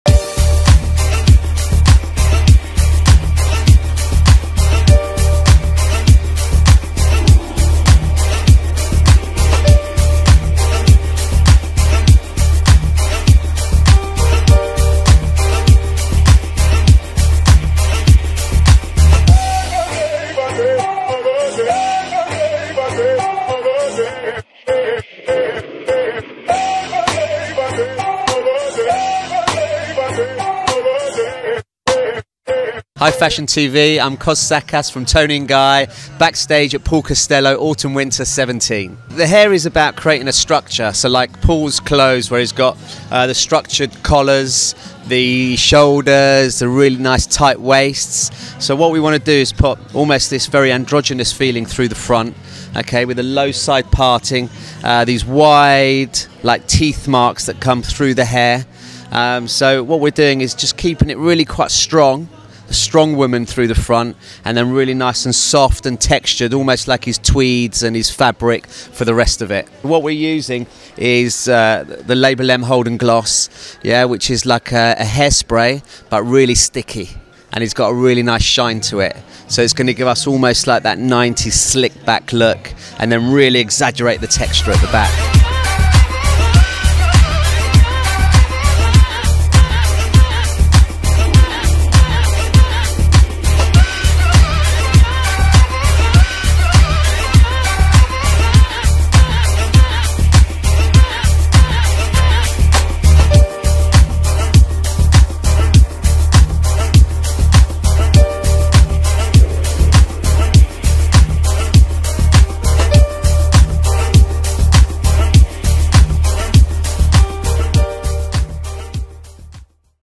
London Fashion Week Fall/WItner 2017-18 - Paul Costelloe Hairstyle | FashionTV
LONDON - The Paul Costelloe hairstyle is about creating a structure to match the Fall/Winter 2017-18 Collection for London Fashion Week. Brought to you exclusively by FashionTV.